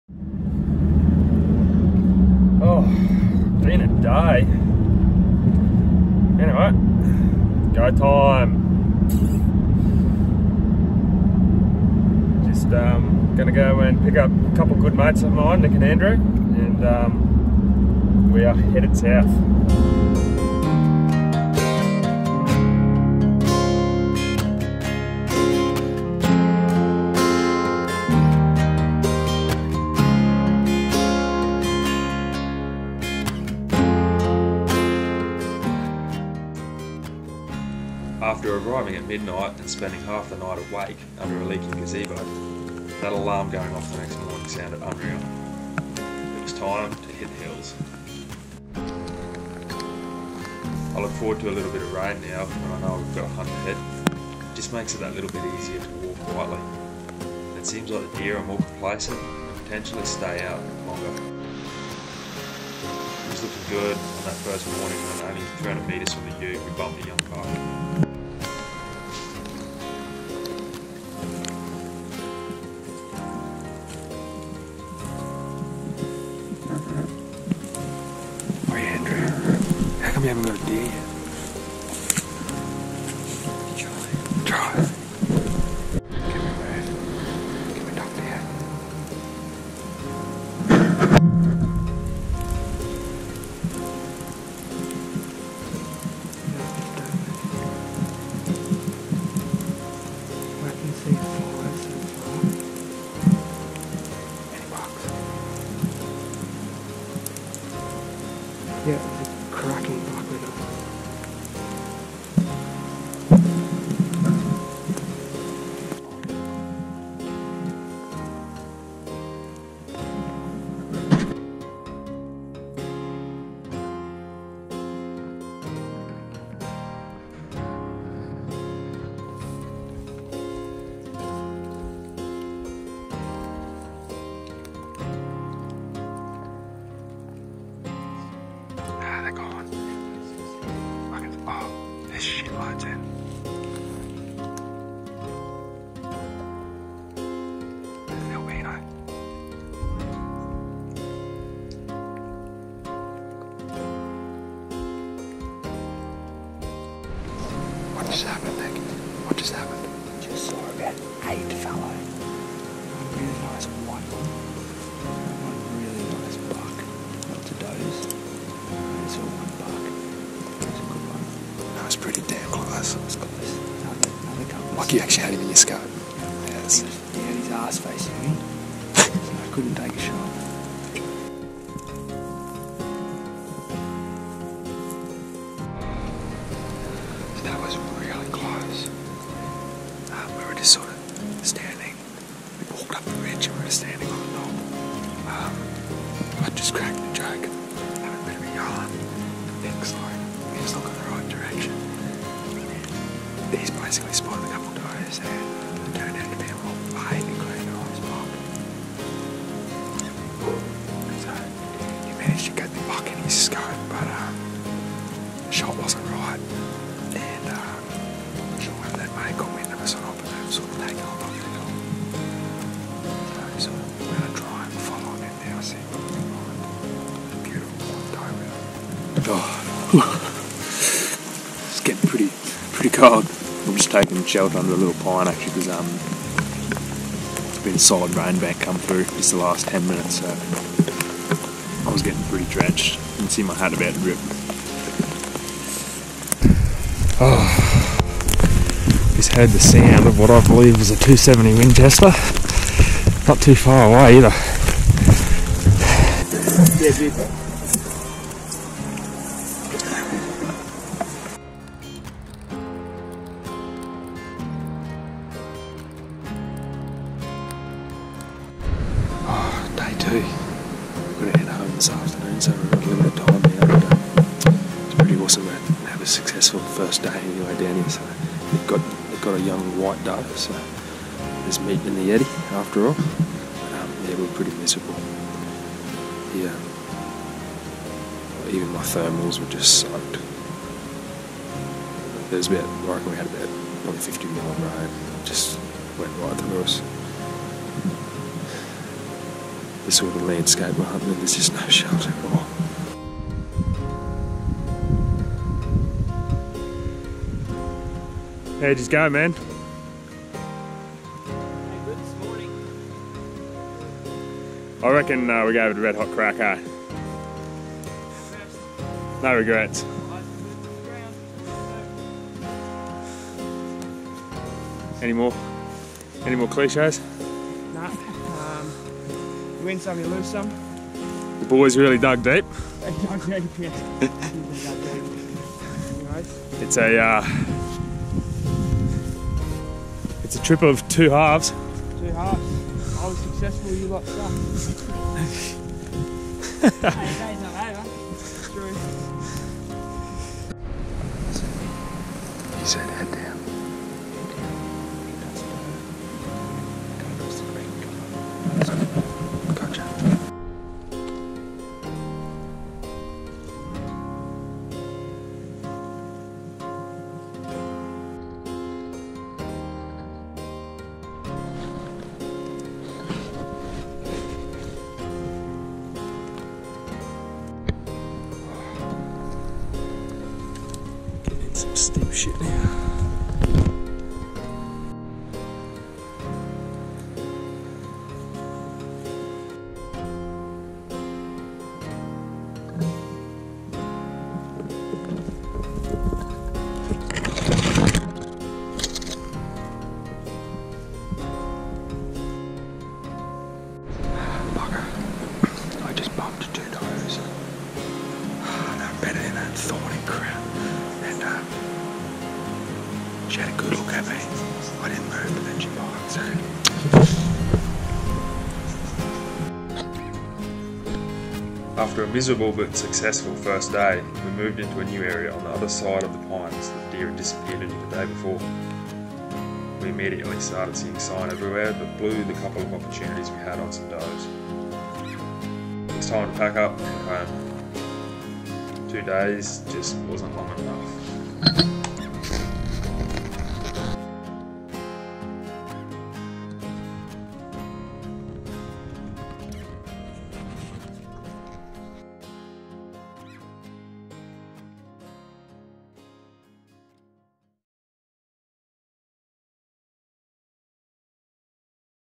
Cold, Wet and Thriving - A NSW State Forest Hunt
A quick mid winter hunt to a NSW State Forest with a couple of good mates. Waaay more rain than anticipated, some early success anyway. Heaps of fallow spotted and ridiculous amounts of sign. We have a few things on the horizon.